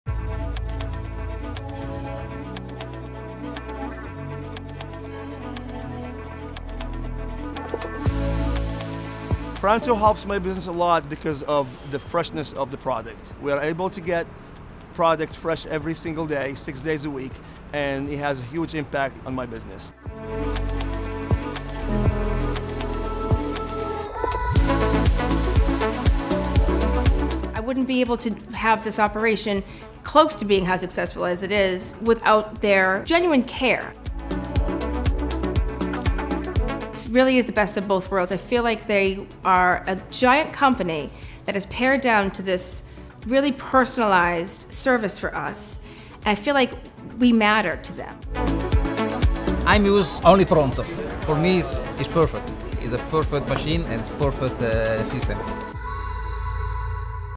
US Foods Pronto: Daily Restaurant Supply Delivery in Urban Areas
Find reliability and the vast selection you would expect from a large supplier, with flexibility that’s customized to meet your specific needs. Get late order times, any order size, and vehicles that can fit your limited space requirements with US Foods® Pronto™.

Pronto is an enhanced service US Foods may provide in your area, offering daily deliveries of the full US Foods product assortment. We’ve helped similar operators get the restaurant supplies they need, delivered fresh daily, so they can provide their customers with the best dining experience and control their inventory.

PRONTO OFFERS:
*Late order times – 8 p.m. cutoff time for next-day delivery
*Delivery six days a week
*No minimum order requirements
*Access to the entire US Foods® innovative food and supplies assortment *Smaller fleet vehicles to accommodate tight spaces